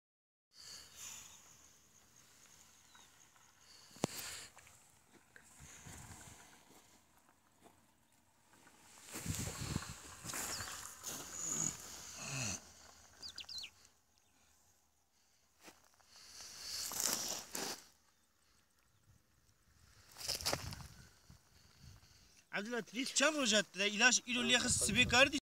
Pamukta Thrips tabaci zararı (Thrips tabaci damage in cotton) (Tropeça em danos tabaci em algodão)
Thrips tabaci, erken dönemde zararlı olan ve vejetasyon süresi boyunca zararı devamı eden bir zararlısıdır. Pamuk yapraklarında kıvrılma, deformasyon ve üst kısımlarda çatallaşmaya neden olmakta ayrıca bitki boyunun kısalmasına neden olarak hasadın gecikmesine de sebebiyet vermektedir (Thrips tabaci is a pest that is harmful in the early period and continues harm throughout the vegetation period. Curl in cotton leaves causes deformation and bifurcation in the upper parts, and also causes shortening of the harvest by causing shortening of the plant height.)